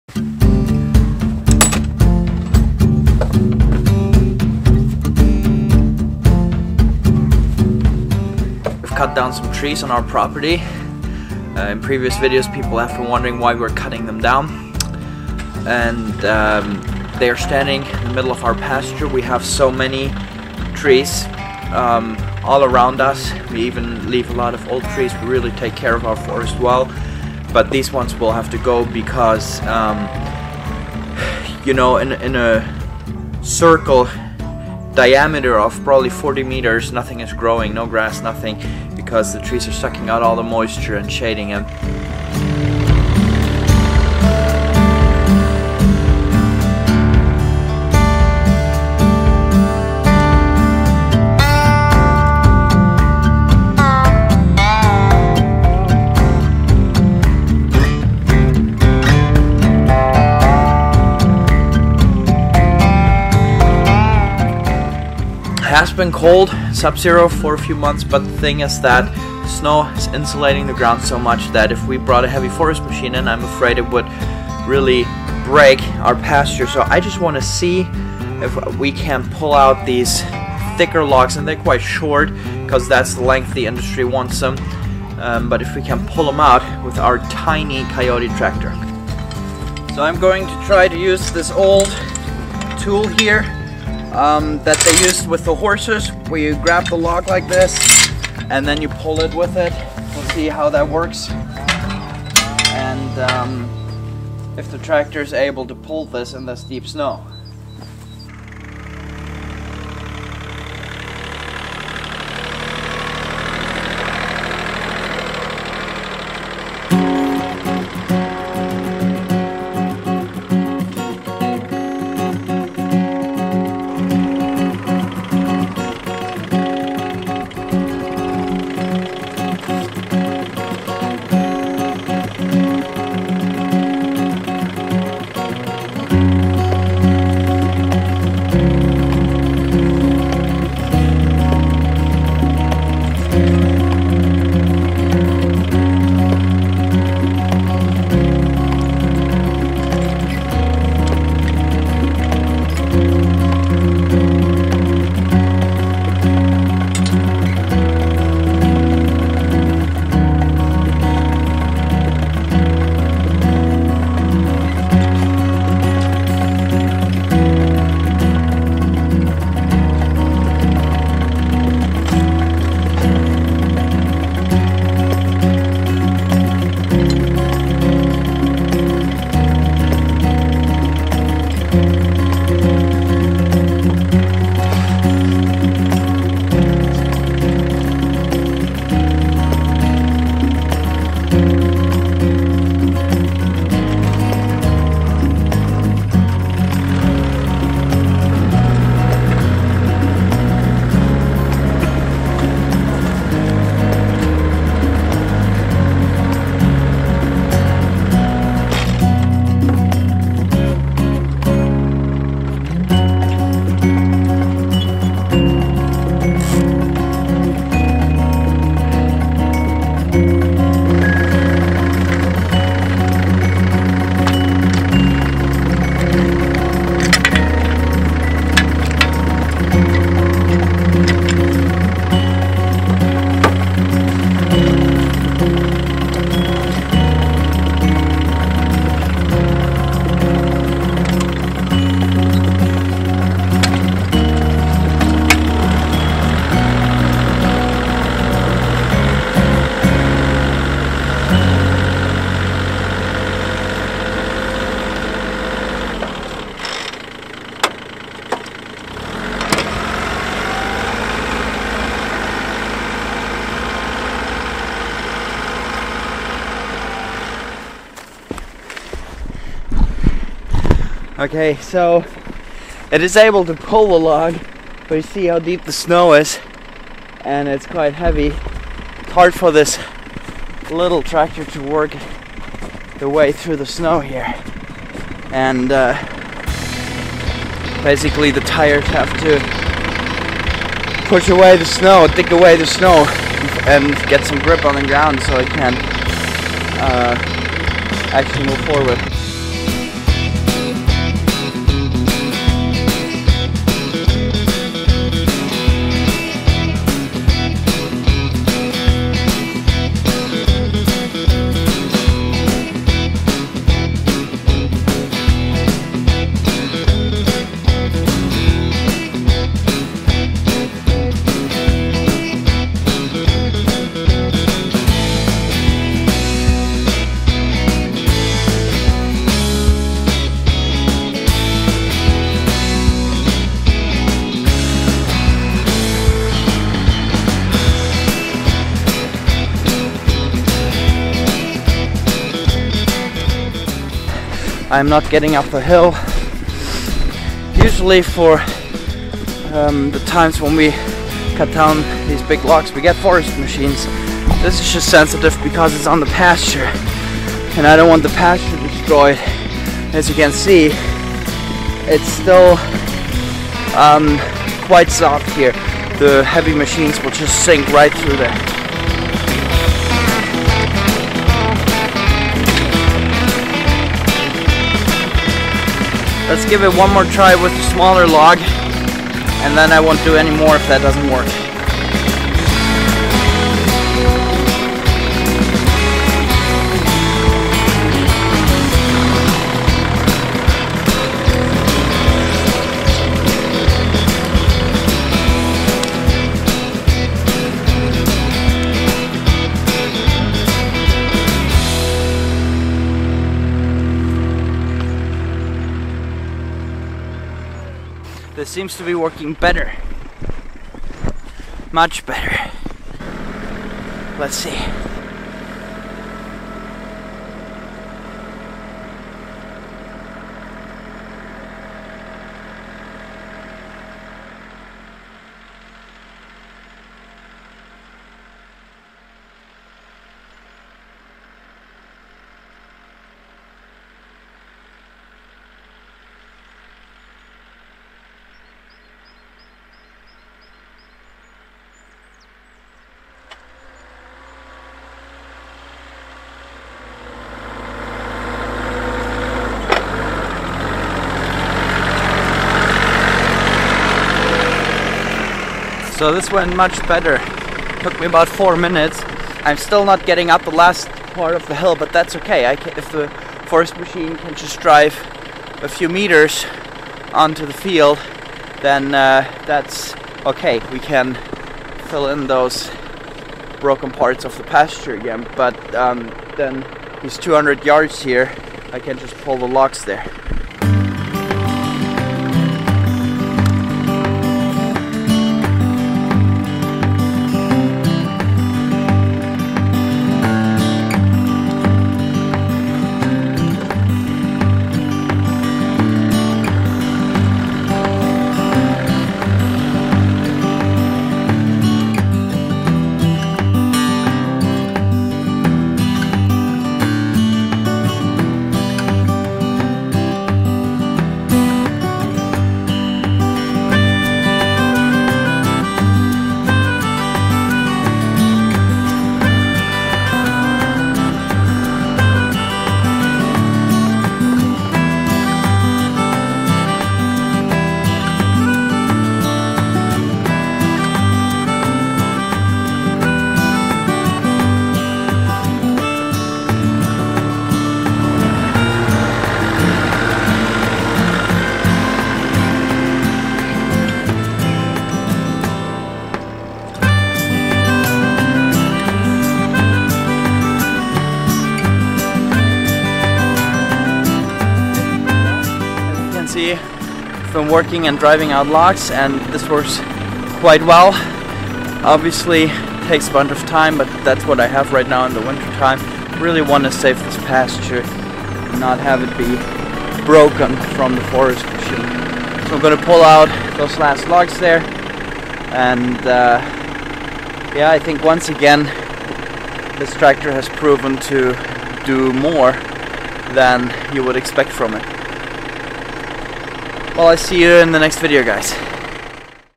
Logging With A Mini Tractor
I don't want the forest machine to destroy the pasture when it will pick up the logs. That's why I try to pull them out with our tiny compact tractor.

Poultry Equipment